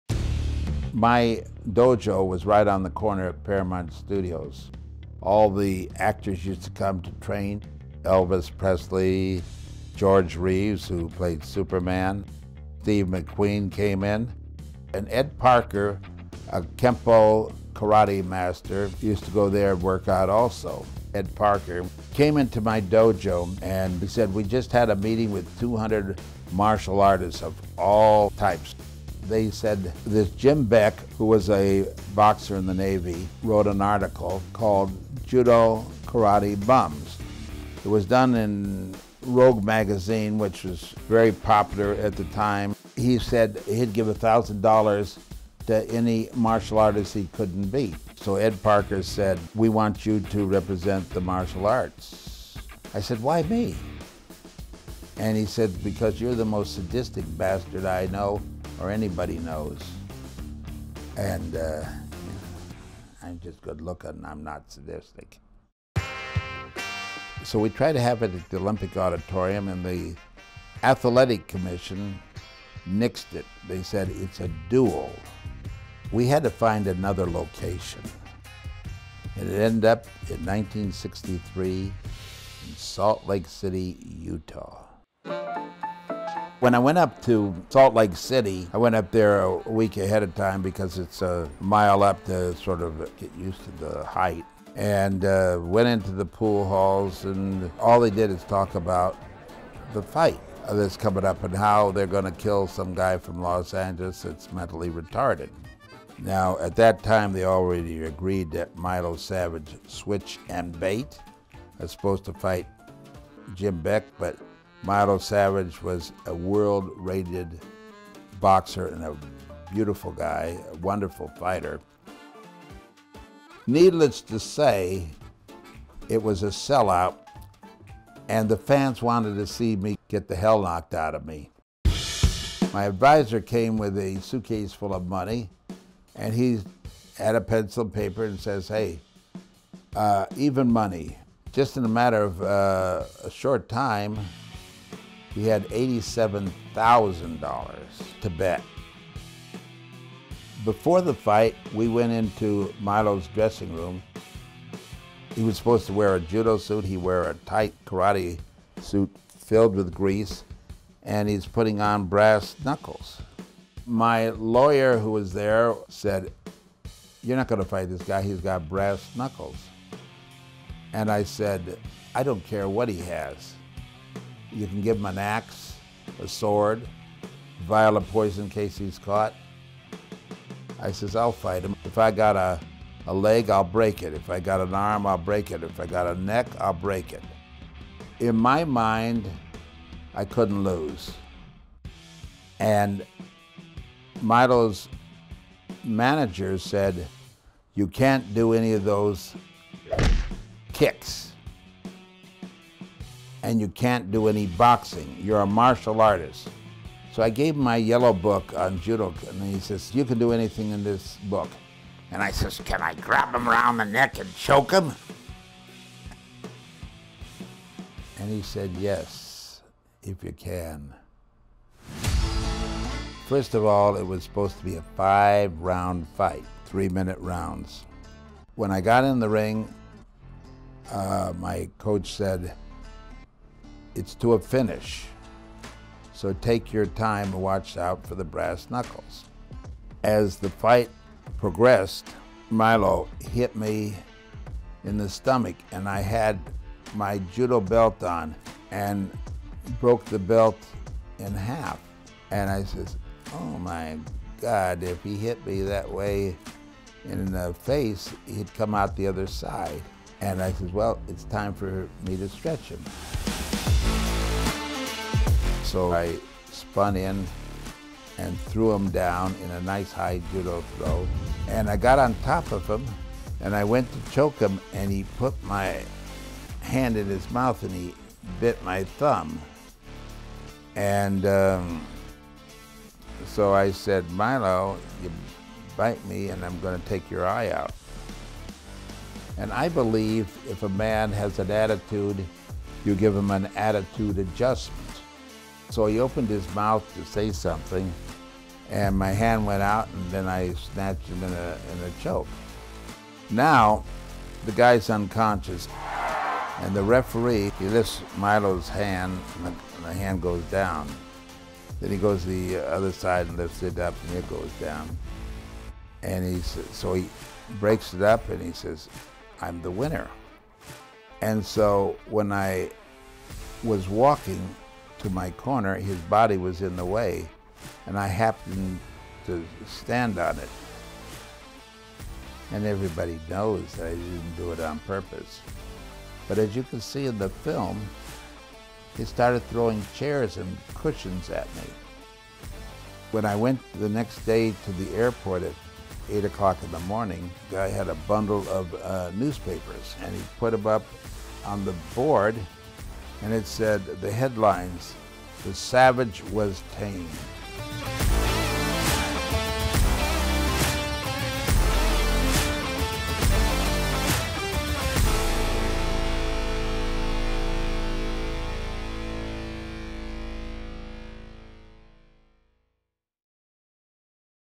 GENE LEBELL FEATURING JUDO VS BOXING FIRST TELEVISED MMA EVENT 1963
A highly personalized and entertaining interview with Judo Gene describing the events leading up to, during and after fight night. The original concept was to work on a documentary or a short film of Judo Gene's life with a working title "The Boy with Red Hair". Others have spoken about such a project, but no one ever went the distance mainly because of the costs of production. We spent many hours with Gene interviewing him and he didn't follow the script, but it was okay, he volunteered a plethora of unabashed comical moments with colorful expletives from his years in the fight and movie biz that had to be edited for public consumption.

There is so much history about Judo Gene that has never been brought to light, yet possibly some day his story can be told. This short video will hopefully give the viewer a glimpse of Judo Gene's personality and entertaining wit.